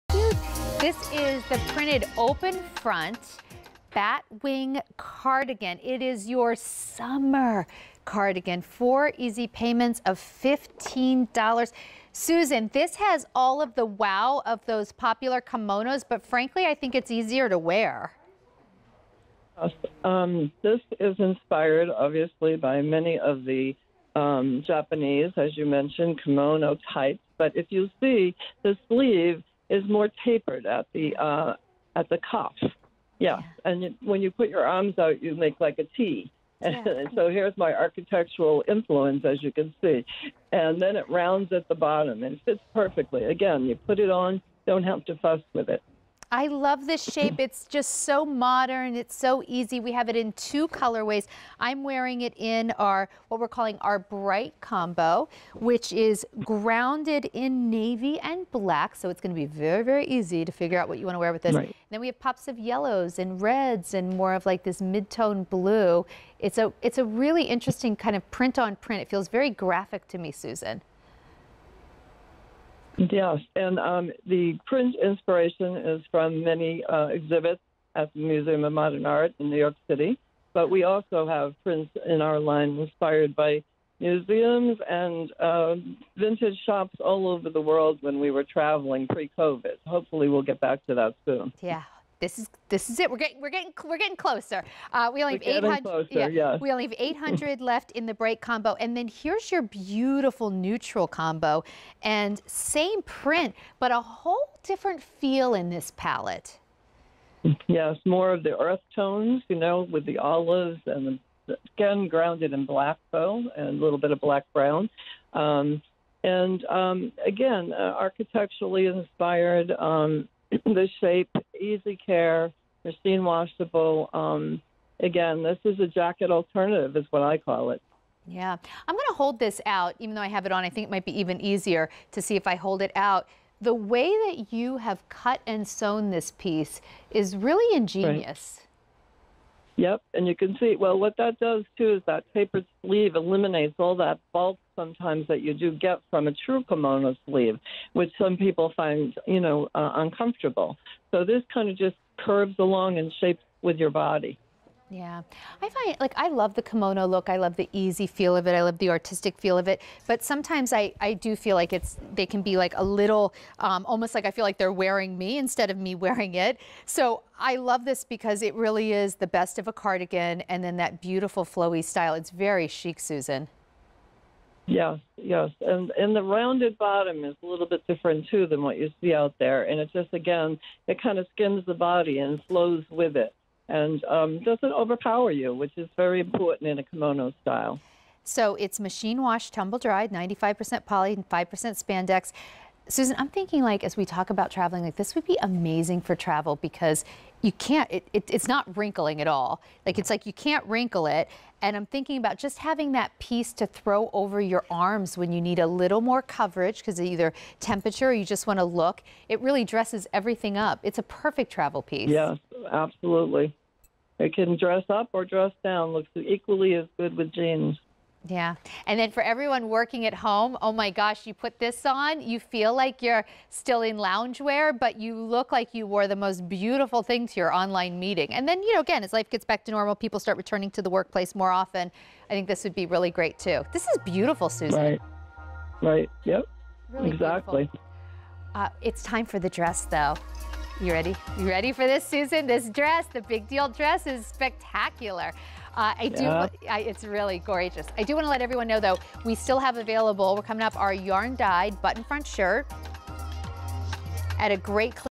Truth + Style Printed Open Front Bat Wing Cardigan on QVC
Truth + Style Printed Open Front Bat Wing Cardigan

This cocoon-shaped cardigan covered with an artistic print provides a lightweight layer of comfort that will transform your everyday outfits to something extraordinary. From Truth + Style.